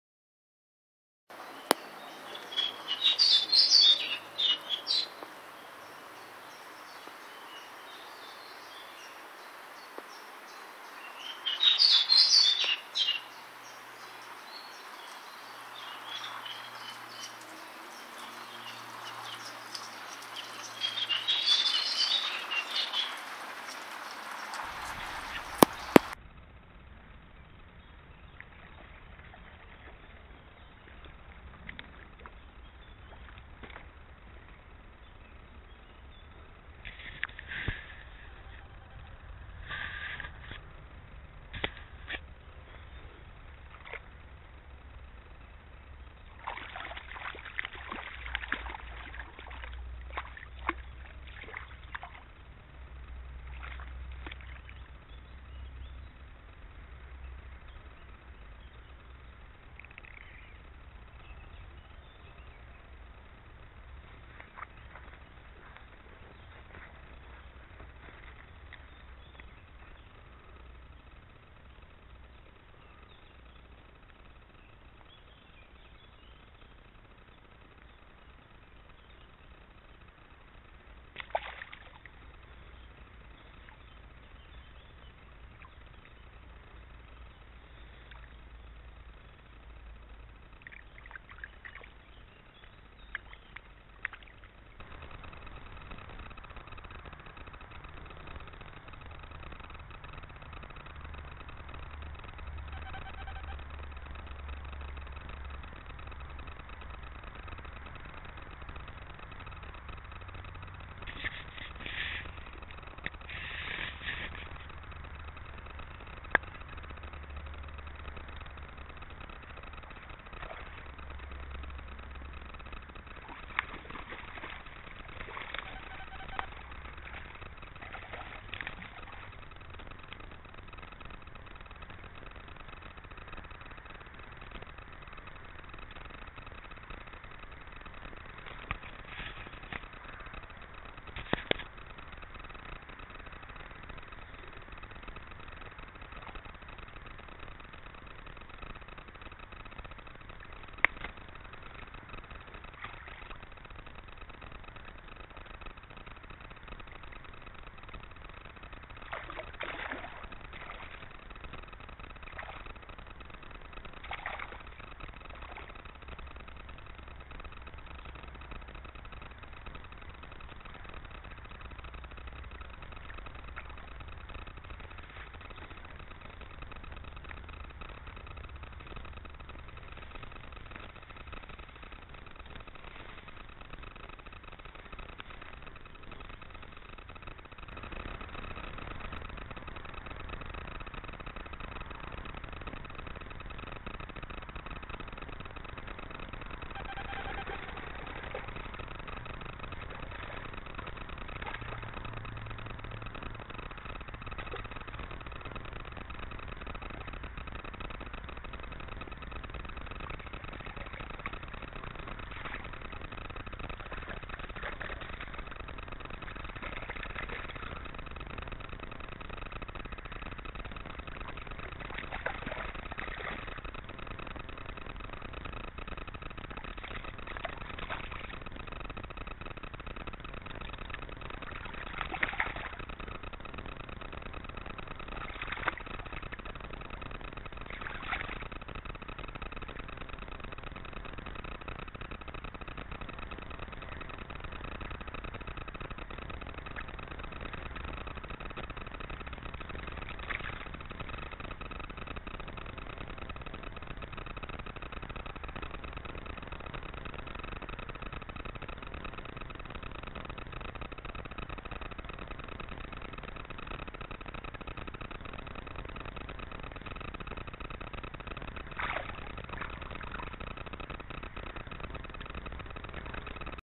pstrągi z małej rzeczki
Podkrakowski ciurek i zabawa z kropkami. Miłego oglądania.